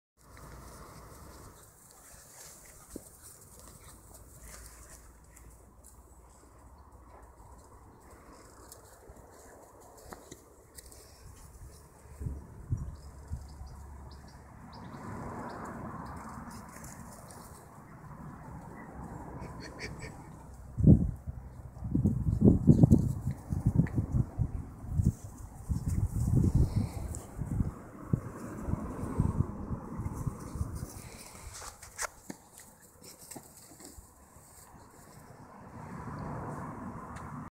Episode Rabbit Ball
Maisie discovers the delights of a rabbit fur lined ball (RIP rabbit) and kills it again..